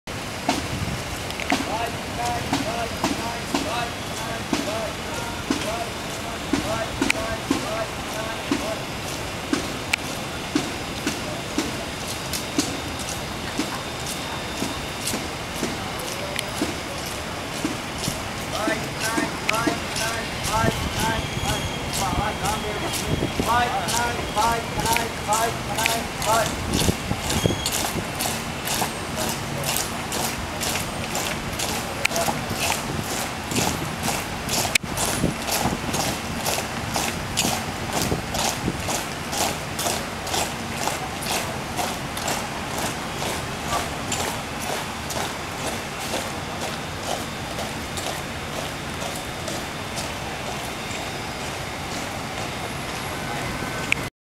Maldives 2009 - Male
Troop review and flag of Maldives in front of the President´s office in Male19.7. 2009